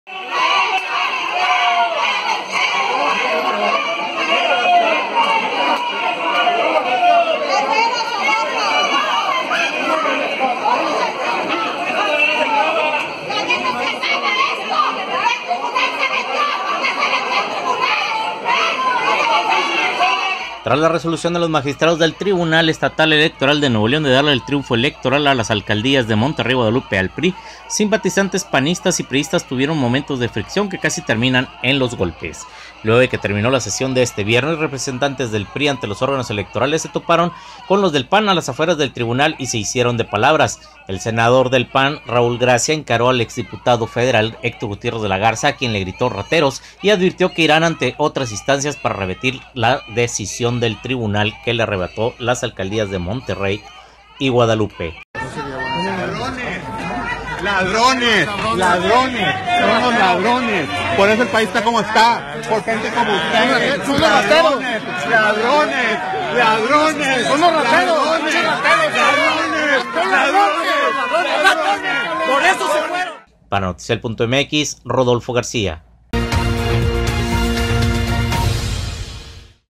Se registran conatos de bronca en Tribunal en Monterrey
Después de fallo que da triunfo al PRI en Monterrey y Guadalupe, panistas y priístas se dan de empujones